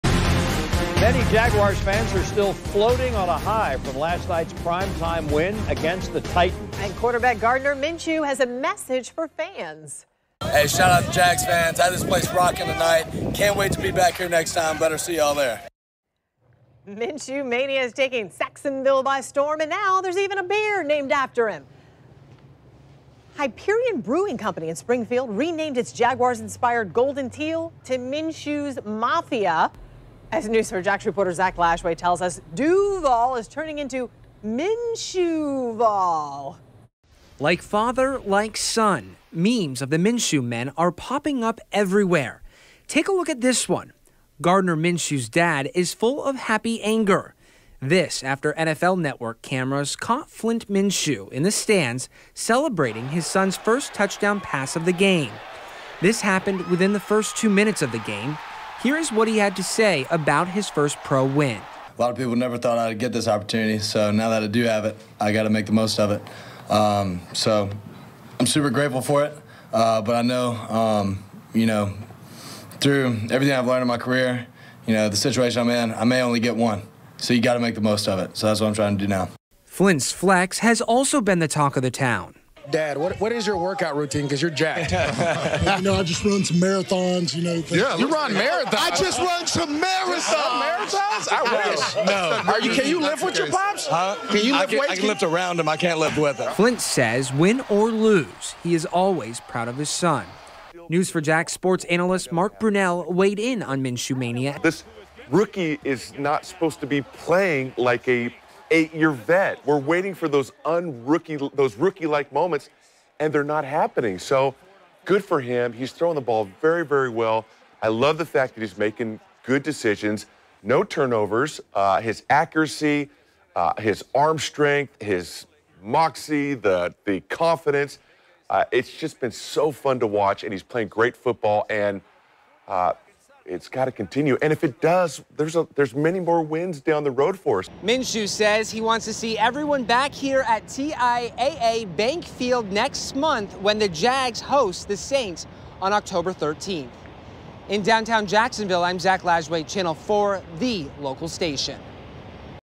Minshew Mania hits fever pitch as QB leads Jaguars to victory
Fans, NFL experts, local businesses all backing rookie quarterback.